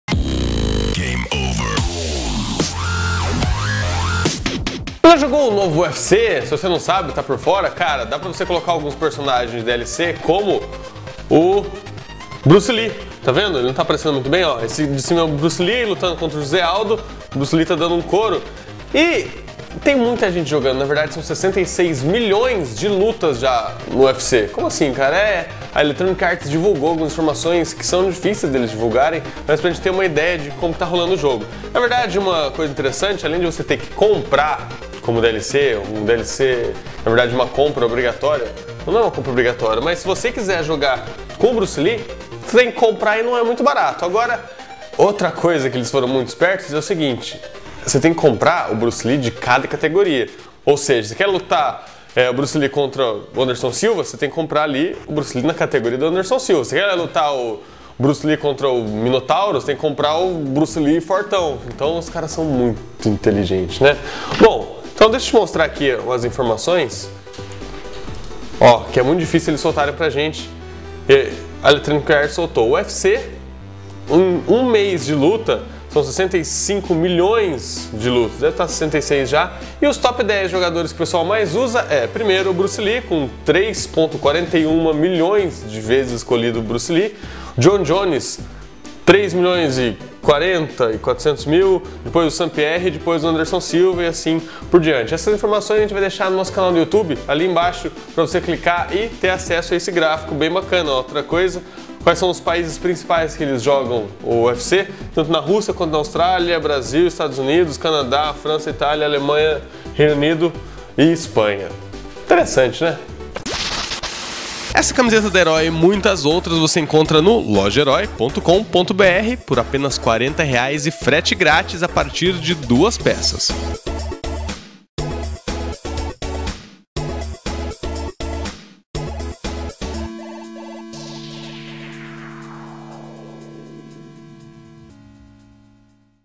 Bruce Lee é o mais popular no UFC - VC VIU?
Matéria sobre o jogo UFC
Apresentação: Arthur Ribas e Thiago Ribas